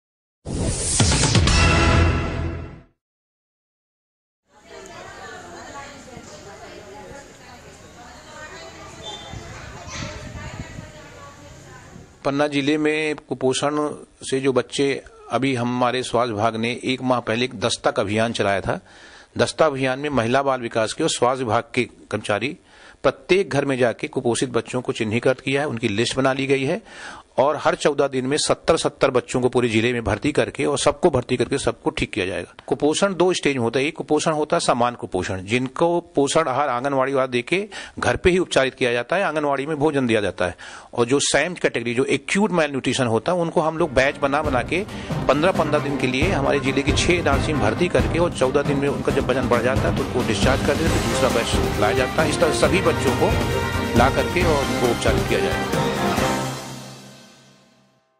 पन्ना- कुपोषण के चंगुल में 17 हजार बच्चे | India First
पन्ना- कुपोषण के चंगुल में 17 हजार बच्चे
- महिला बाल विकास और स्वास्थ्य विभाग की हकीकत
- कुपोषित बच्चों को आनन फानन में किया अस्पताल में भर्ती
- कुपोषण मुक्त मध्यप्रदेश को पलीता लगा रहे अफसर
- कुपोषण के नाम पर करोड़ो के बजट का आखिर कहां हो रहा उपयोग

- दस्तक अभियान की सर्वे रिपोर्ट का खुलासा
- कुपोषण मुक्त करने के नाम पर सरकारी खजाने का कहां हो रहा उपयोग
- पोषण पुनर्वास के नाम पर खानापूर्ति कर रहा विभाग
- जिम्मेदार आदतन आश्वासन से कुपोषण मिटाने का कर रहे दावा